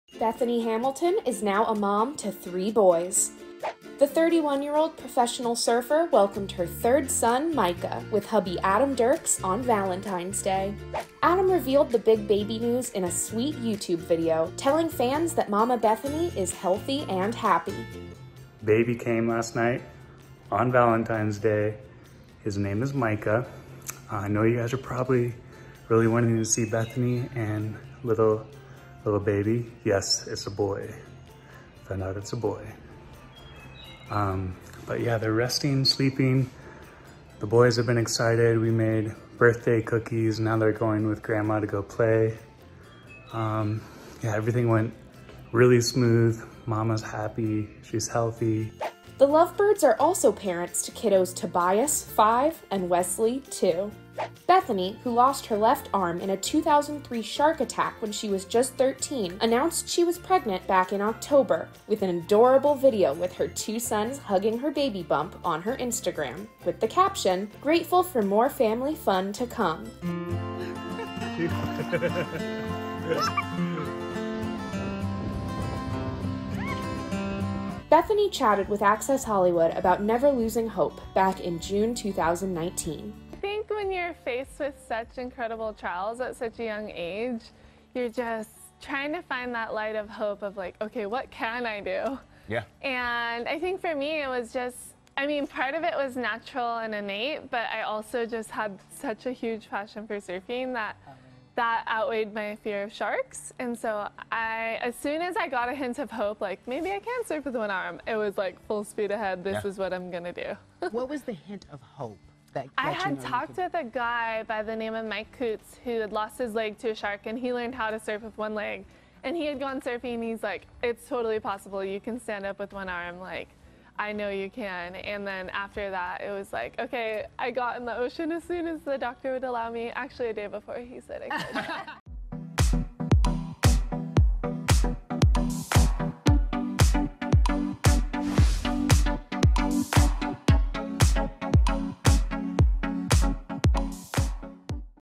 Bethany Hamilton Gives Birth To Third Son
Bethany Hamilton is now a proud mom to three boys. The 31-year-old welcomed her third son, Micah, with hubby Adam Dirks on Valentine’s Day. Adam revealed the news in a sweet YouTube video telling fans that the professional surfer who lost her left arm in a shark attack in 2003 is healthy and happy. The couple are also parents to son Tobias, 5, and Wesley, 2.